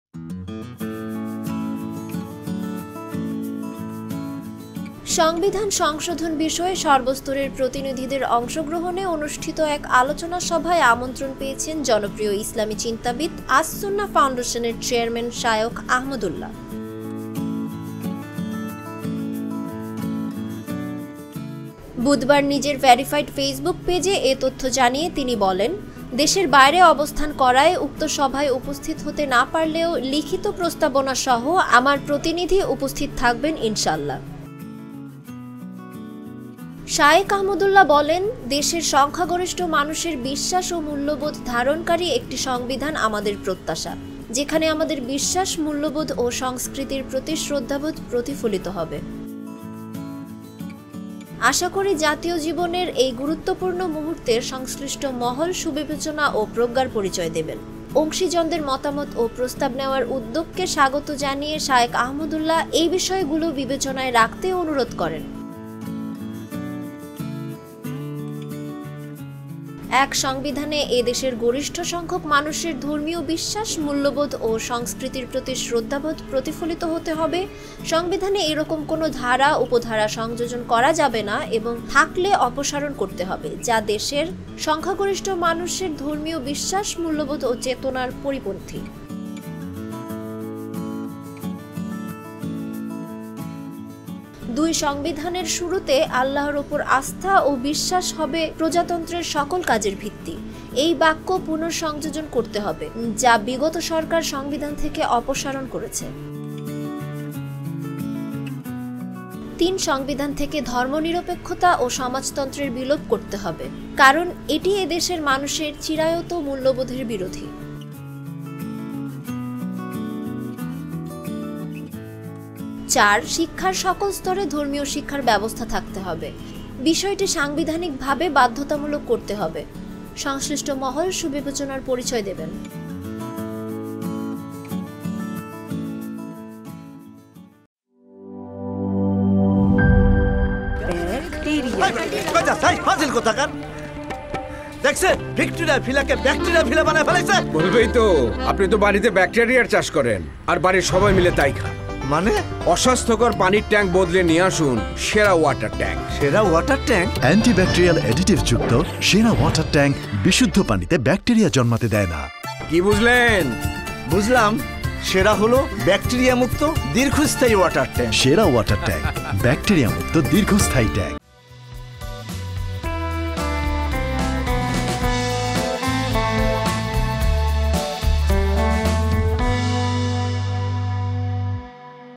সংবিধান সংশোধনে ৪ প্রস্তাবনা দিলেন শায়খ আহমাদুল্লাহ | Sheikh Ahmadullah
সংবিধান সংশোধনে ৪ প্রস্তাবনা দিলেন শায়খ আহমাদুল্লাহ